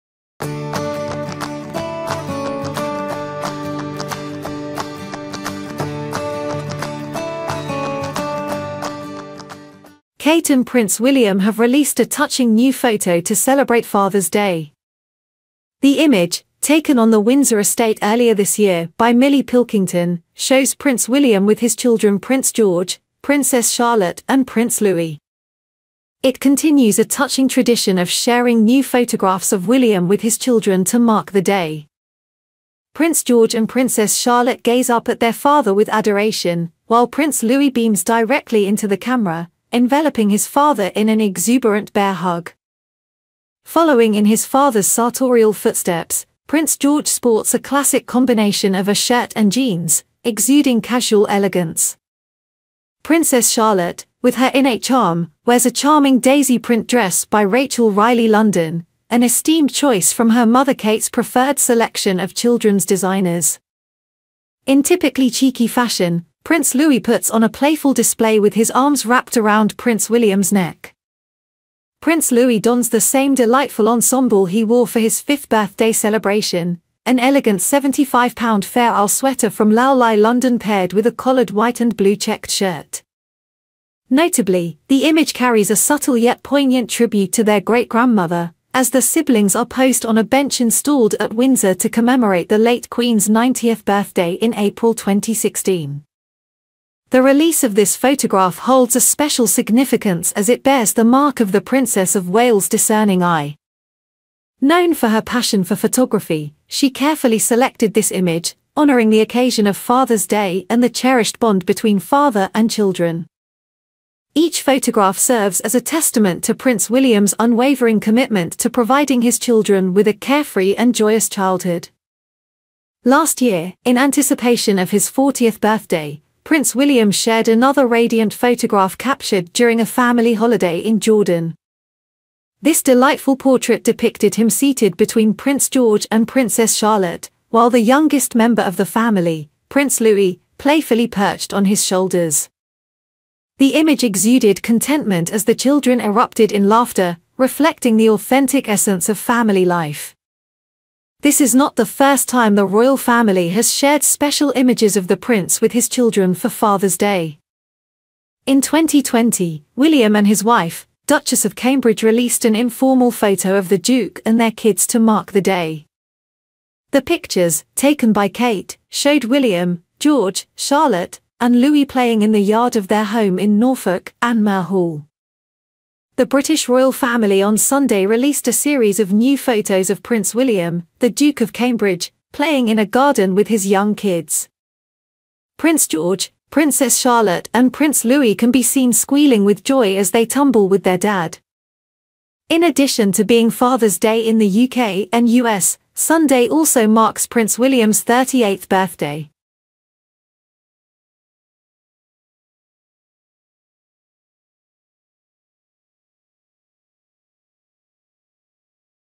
Catherine Emotion As Her Children Grow Up Too Fast In A New Photo To Mark Father's Day
Catherine emotion as her children grow up too fast in a new photo to mark father's day, The image, taken on the Windsor Estate earlier, shows Prince William with his children Prince George, Princess Charlotte and Prince Louis.